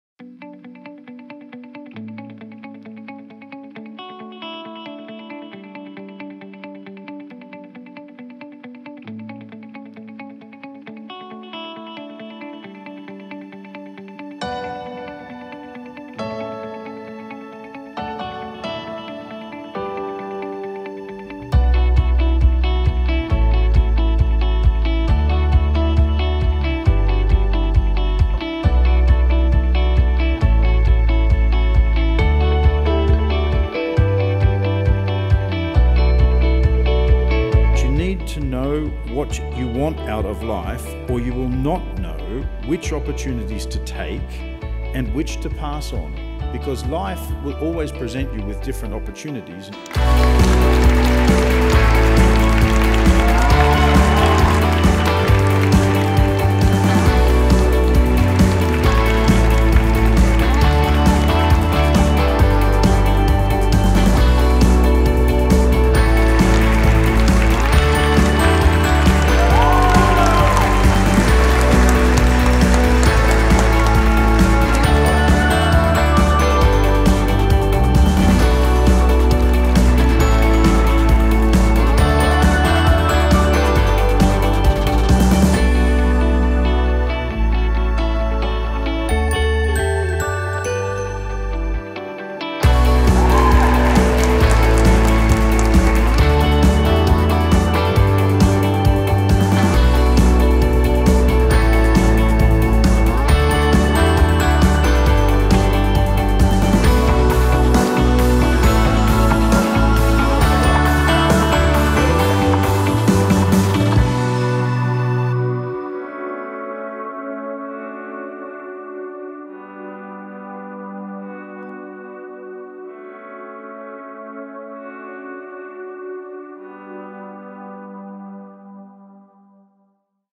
Celebrating the Class of 2019: Graduation in Historic Hofburg
Inside of the beautiful Hofburg Imperial Palace in the heart of Vienna, Webster Vienna Private University’s Commencement Ceremony!